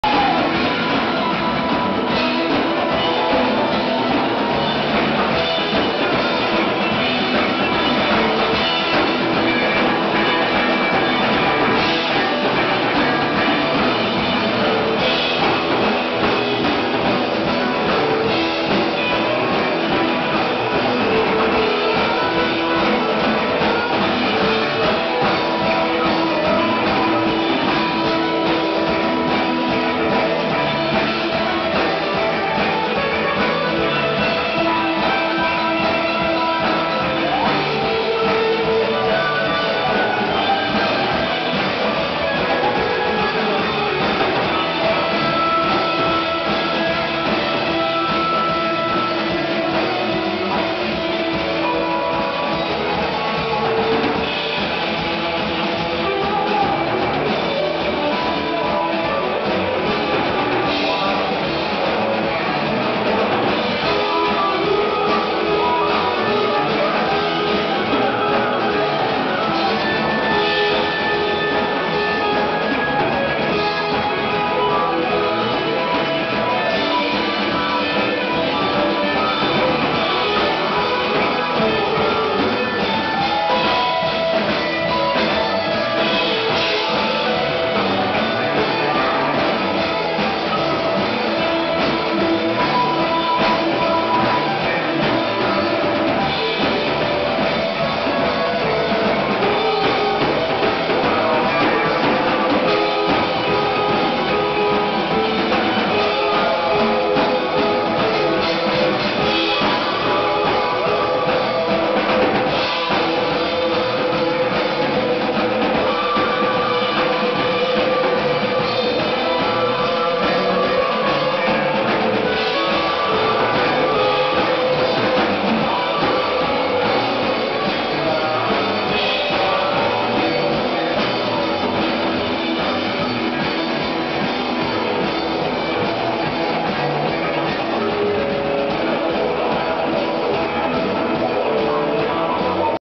The Mayflies as Grateful Dead
Jammin at the Hub in Cedar Falls
10/31/11